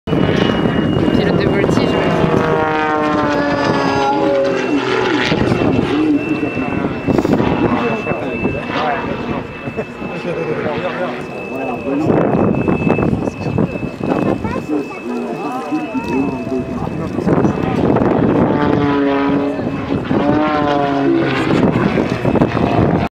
Extra 300 SC video at Cazaux French Air Force base #4
Extra 300 SC piloted by the French Air Force aerobatics team